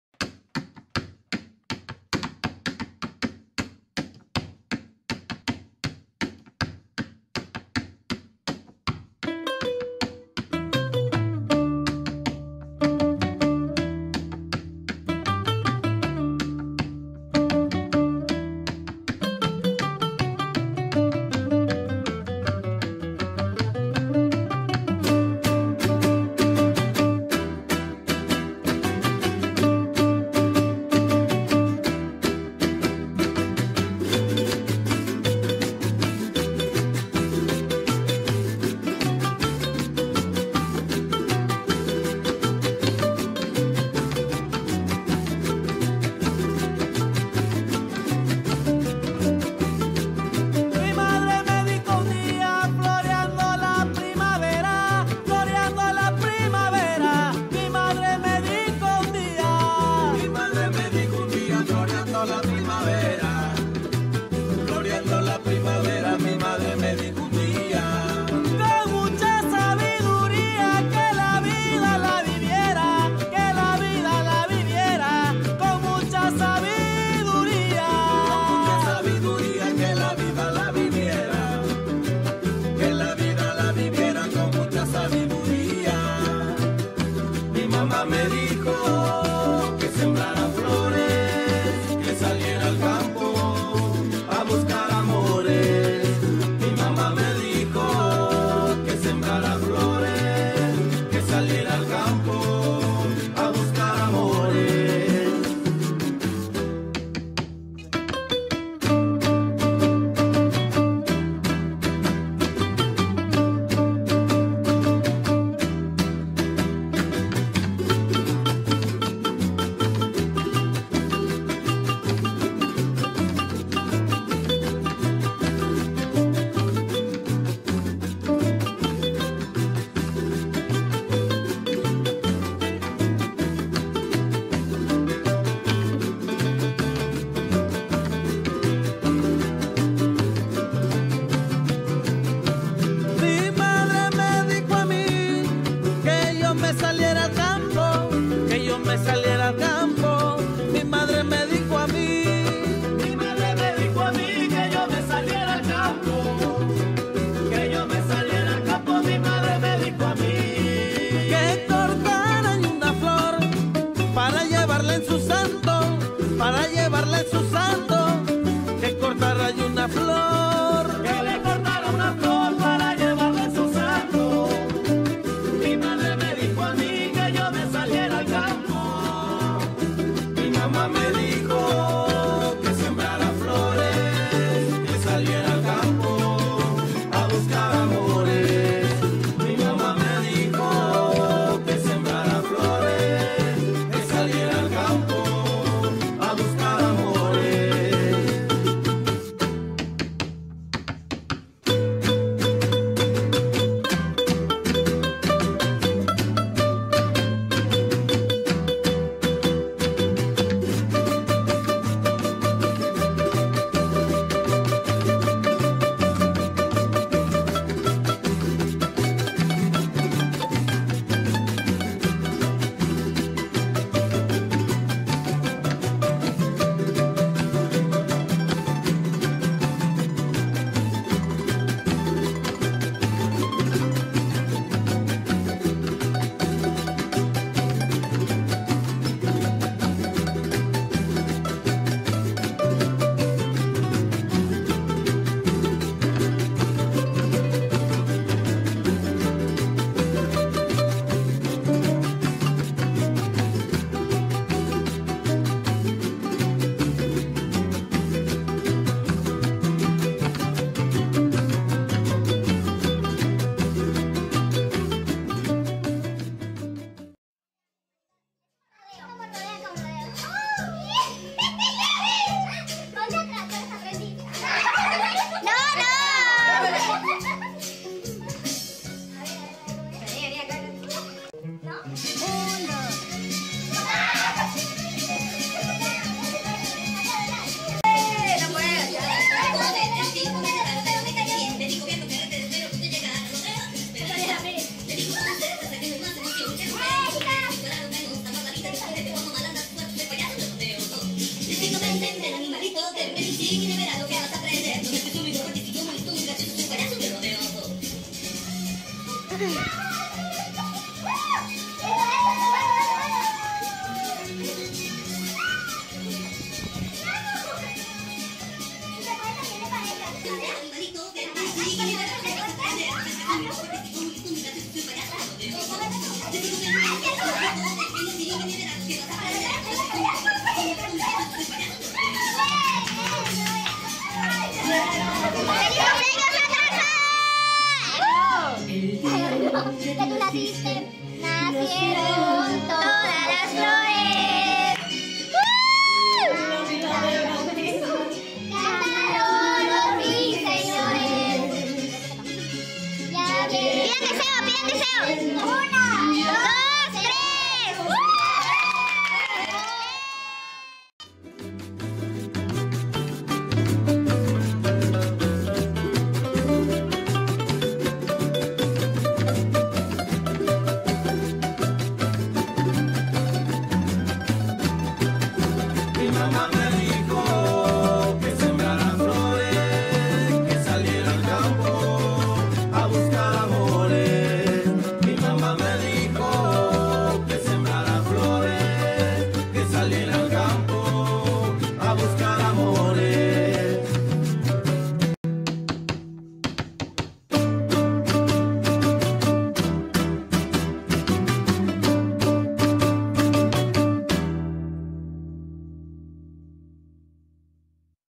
Estudiantes Visitantes 2017-1 | Carne Asada
No podía faltar la carne asada de fin de semana para nuestros estudiantes visitantes 2017-1.
La pasamos de lo mejor con la deliciosa comida, buenísima compañía, intercambio de culturas, música y un pequeño detalle para las cumpleañeras.